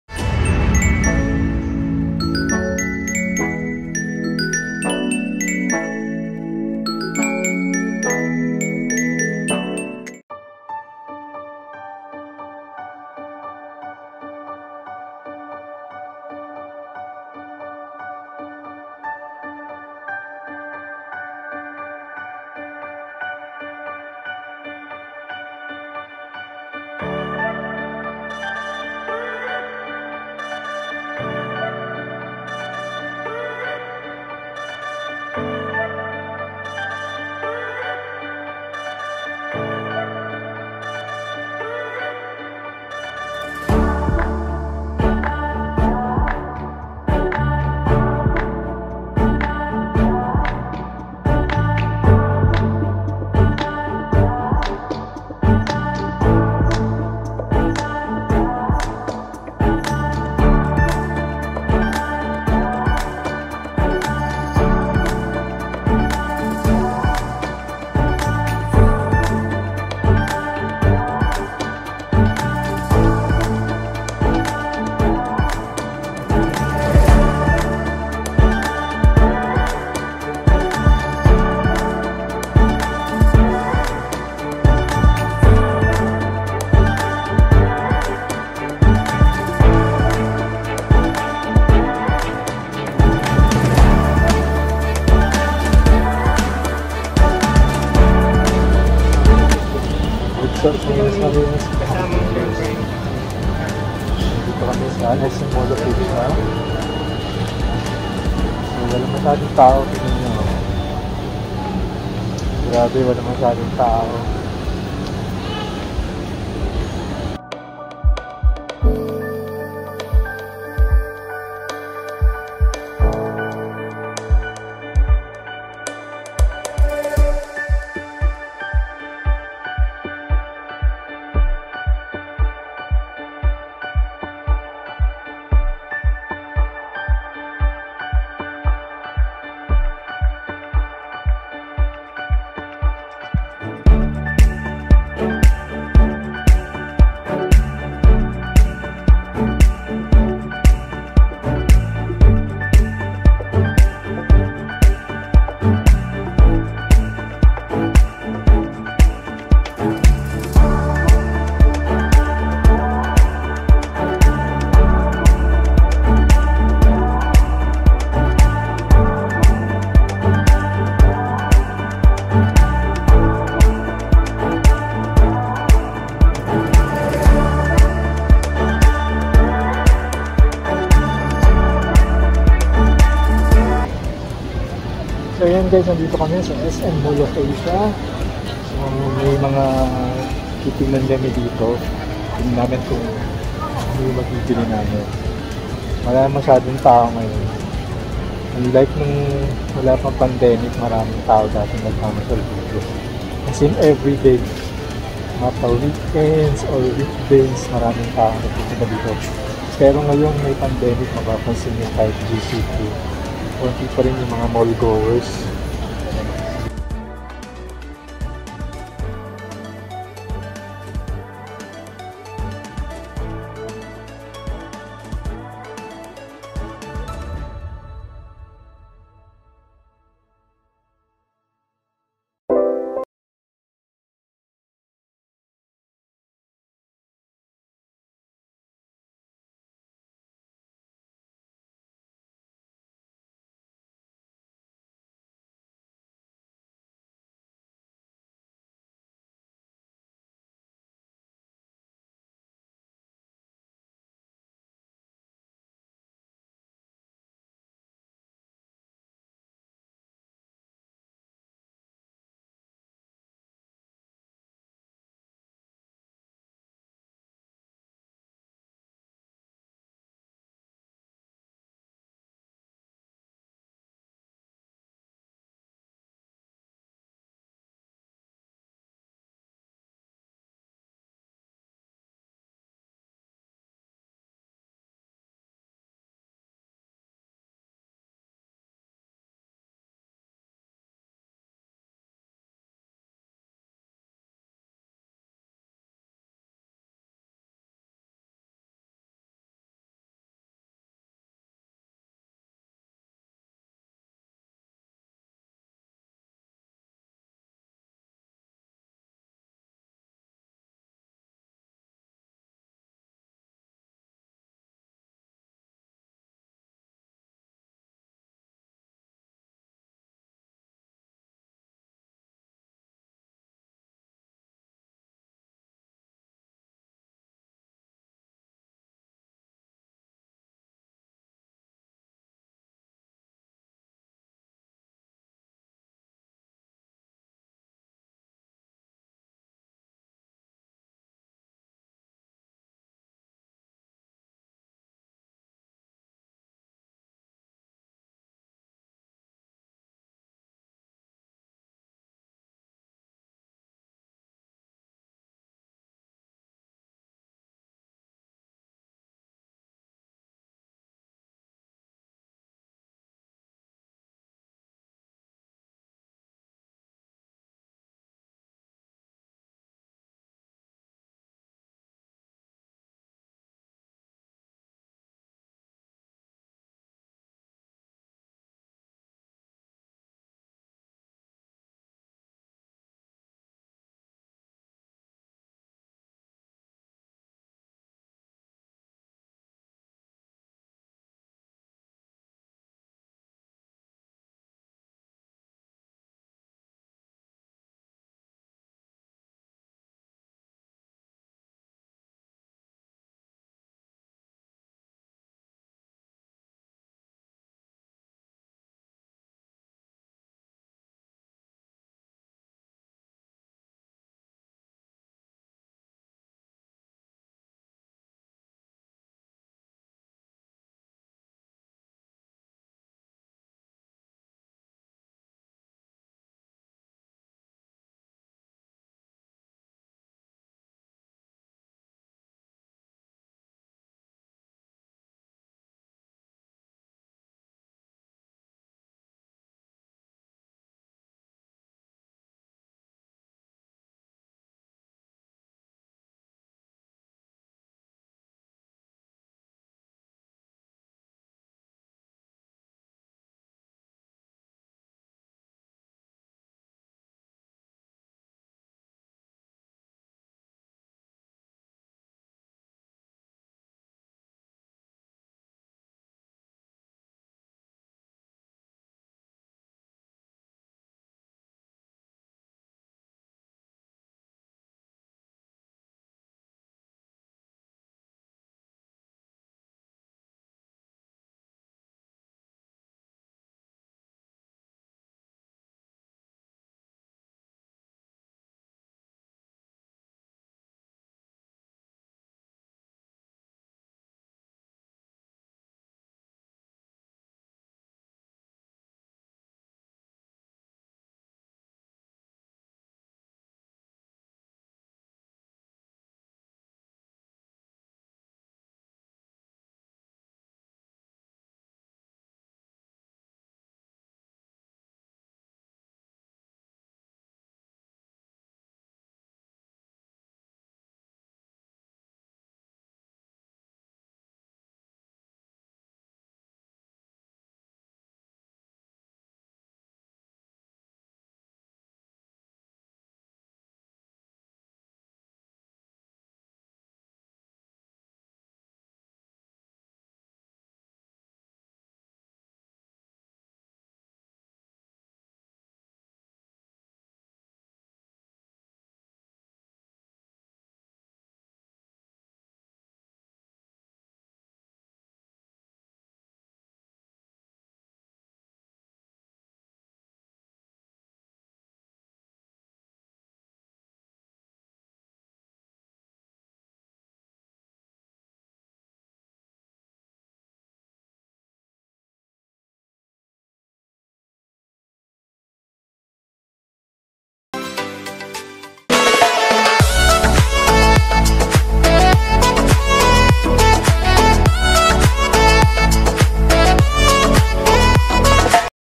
SAMSUNG S20 PLUS CAMERA TEST / VLOG TEST (LOW-LIGHT) AT SM MALL OF ASIA
Samsung Galaxy S20 Plus camera test in low=light.